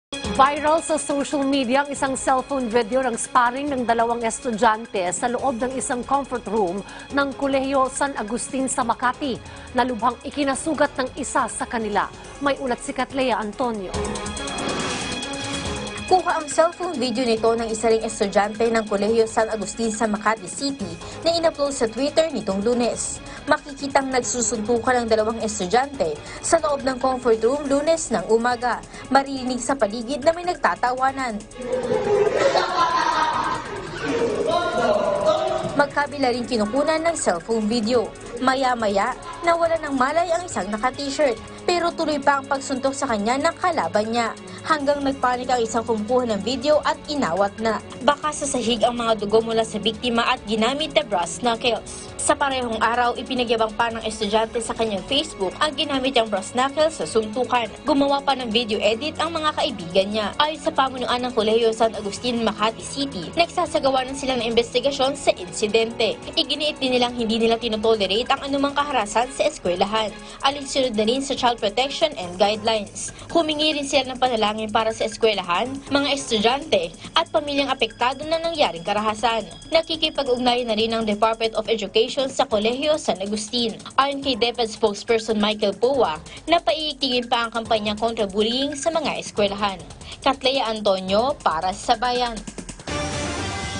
Colegio San Agustin, iniimbestigahan na ang insidente ng pagsusuntukan ng dalawang estudyante...
Colegio San Agustin, iniimbestigahan na ang insidente ng pagsusuntukan ng dalawang estudyante sa palikuran